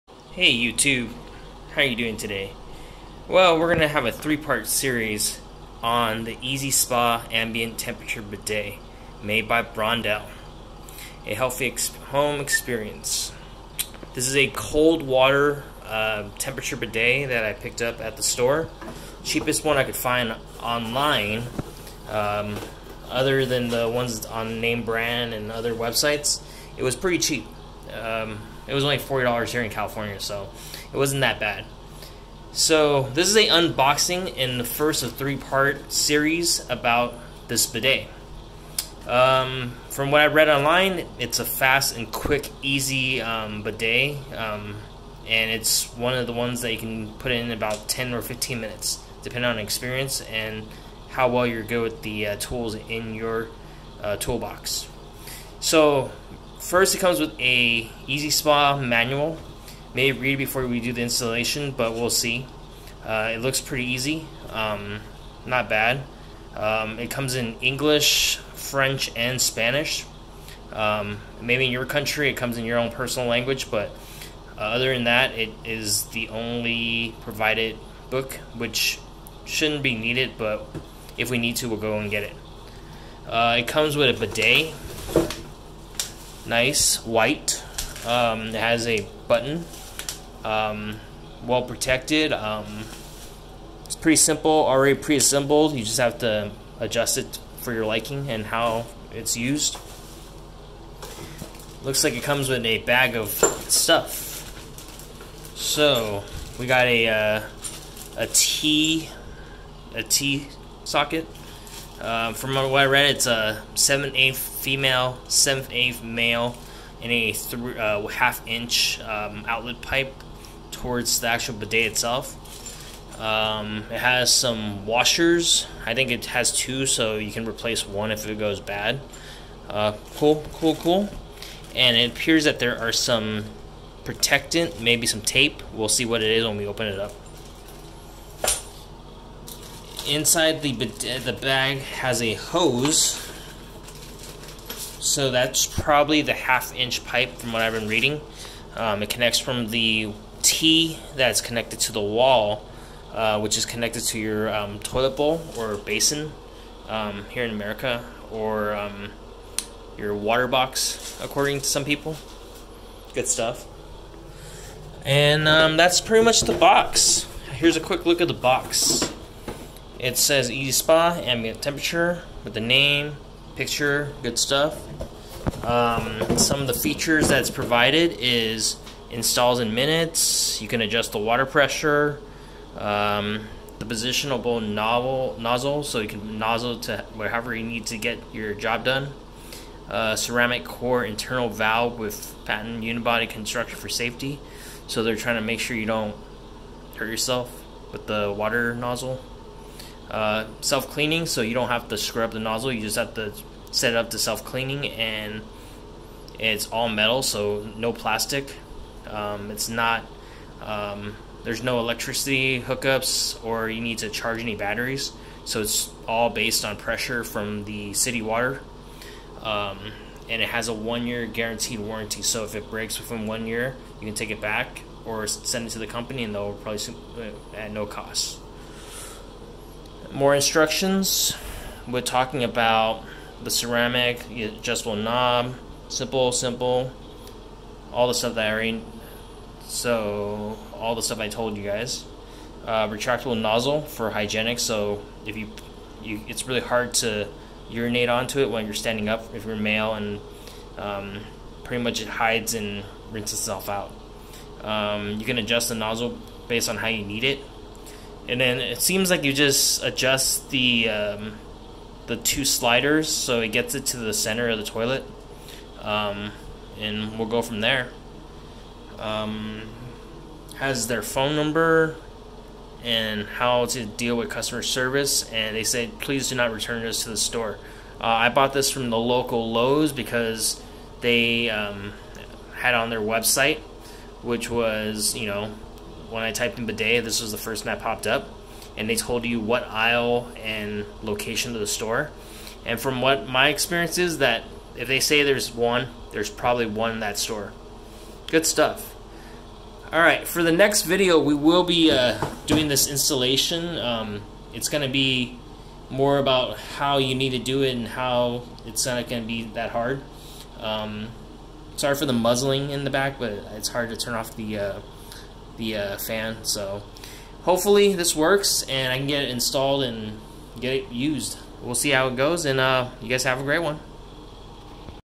Bidet
Unboxing of a bidet by brondell

It does not look too complicated . In this video i unbox a easy spa bidet.

So no toilet paper . no problem.